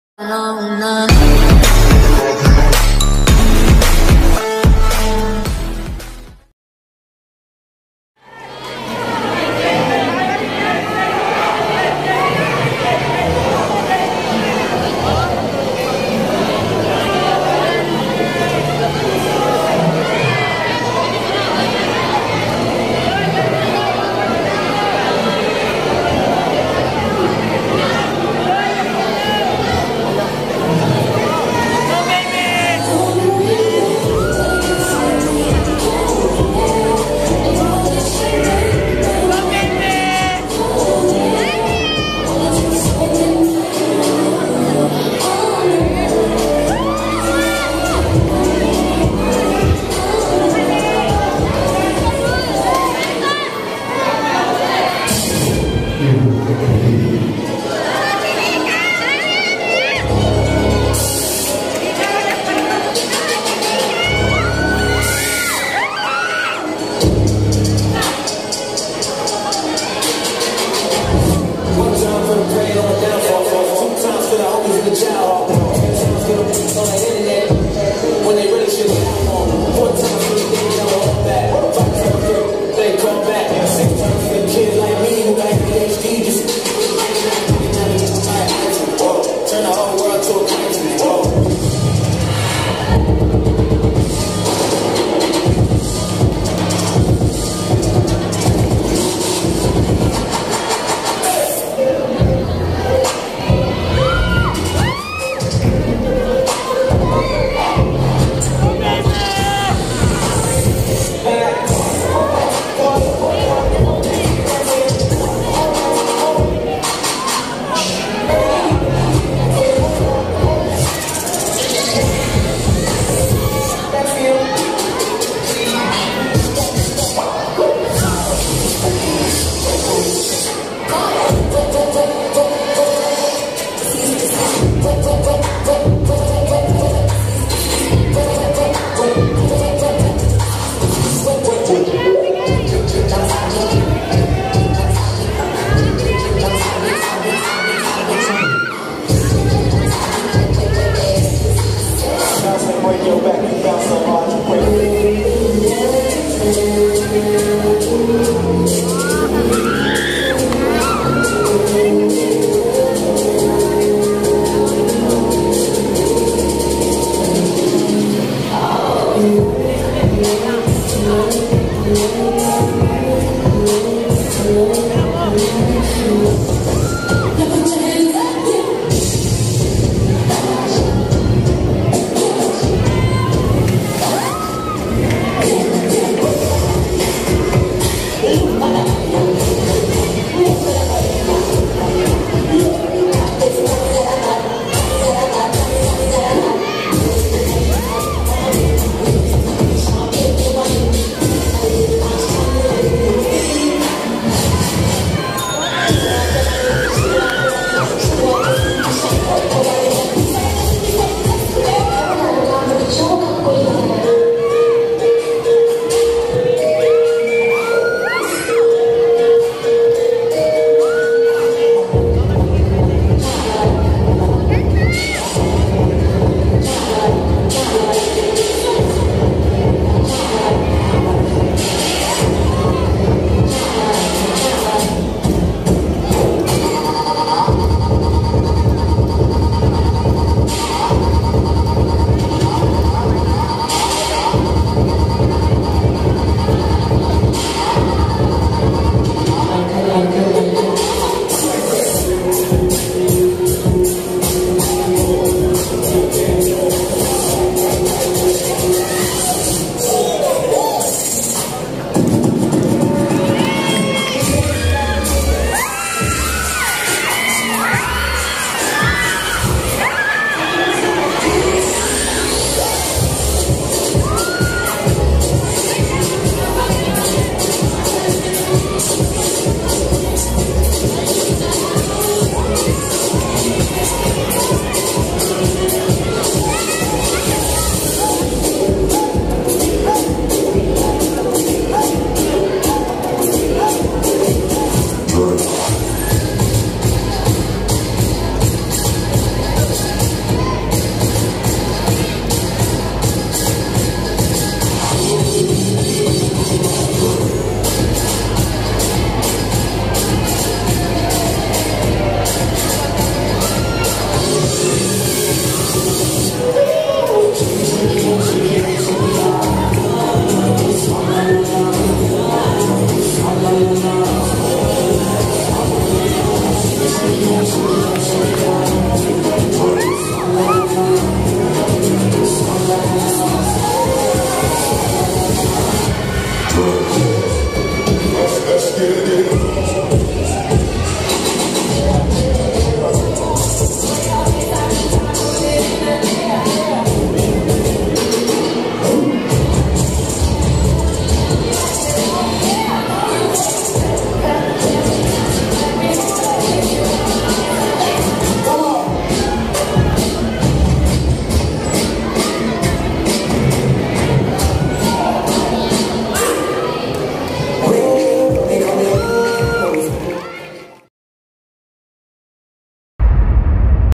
U EXIST DANCE CONTEST BRGY KATIPUNAN Q.C OCT 8,2022
CONGRATS TO ALL
CHAMPION VIVACIOUS
1ST PLACE UNITED AS ONE
2ND PLACE HISTACITY FAM